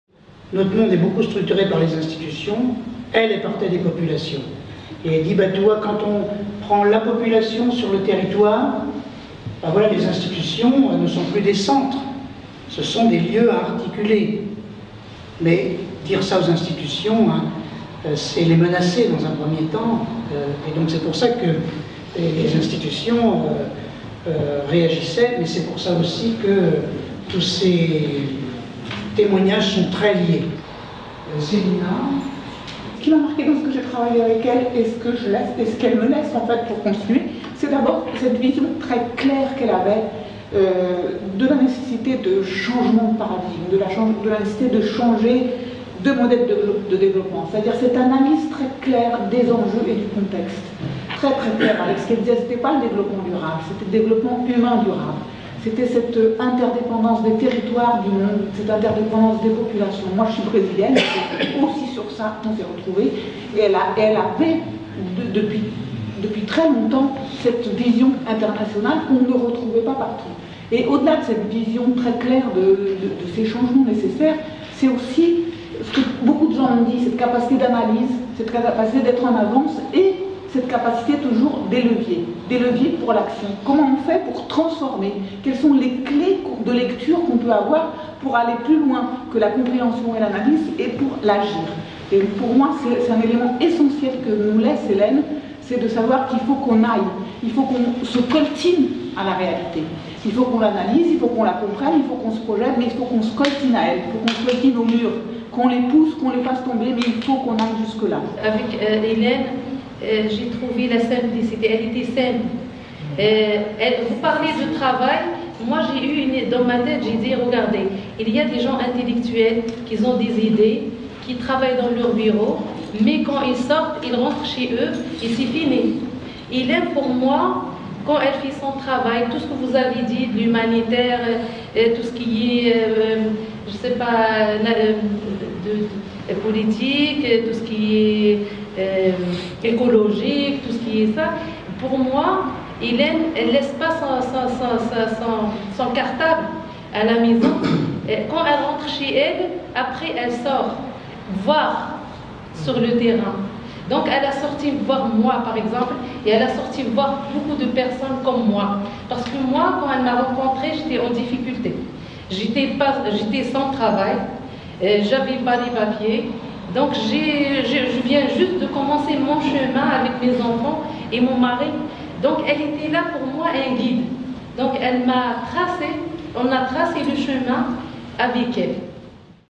Hommage à Hélène Combe - Maurice Loizeau, Célina Whitaker
"Hélène a changé le paradigme du développement durable, et forgé des leviers pour l'action..."

Témoignages de Maurice Loizeau, Célina Whitaker et une participante du public.

Hommage à Hélène Combe - 19 juin 2015
Vidéos, reportage et montage : Pierre Bordais.